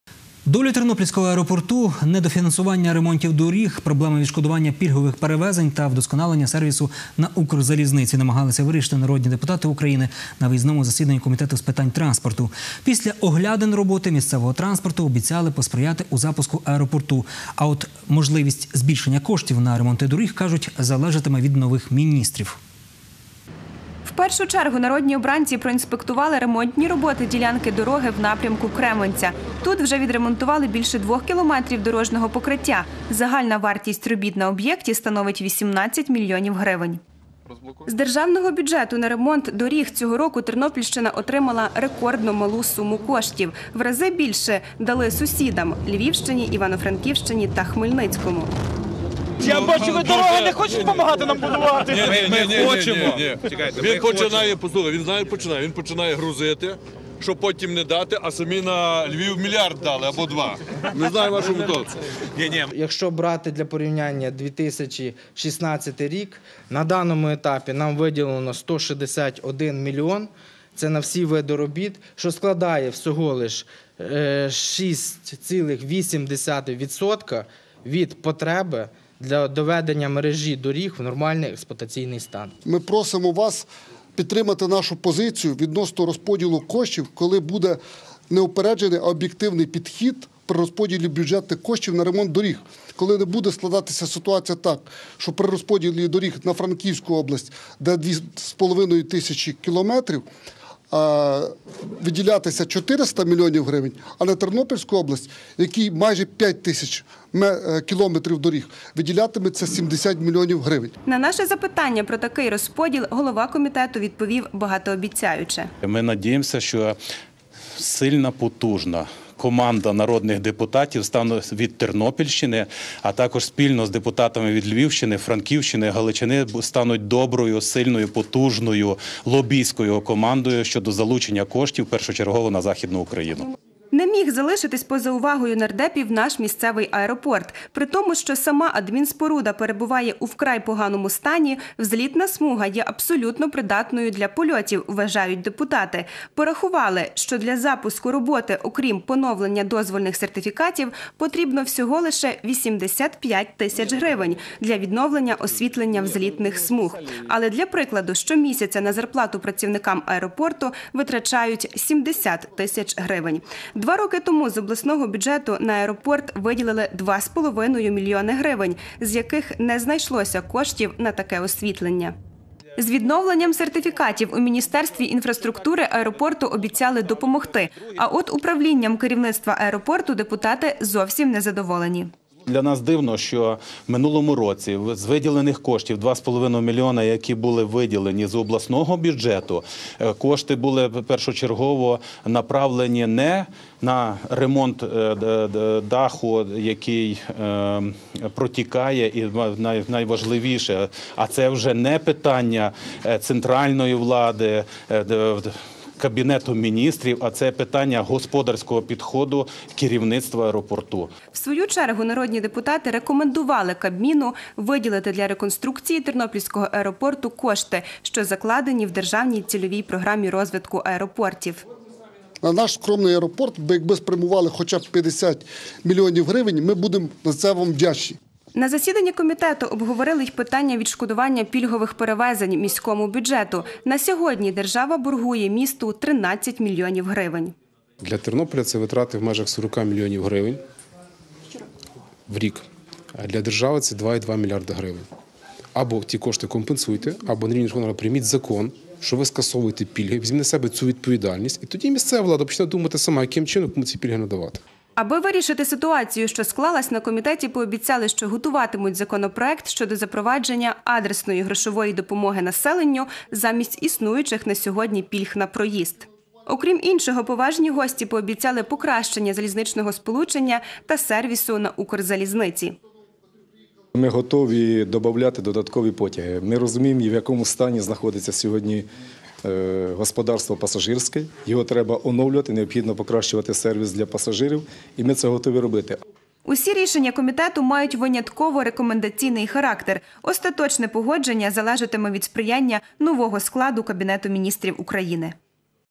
На Тернопільщині відбулося засідання Комітету ВРУ з питань транспорту
Долю Тернопільського аеропорту, недофінансування ремонтів доріг, проблеми відшкодування пільгових перевезень та вдосконалення сервісу на Укрзалізниці намагались вирішити народні депутати України на виїзному засіданні Комітету з питань транспорту.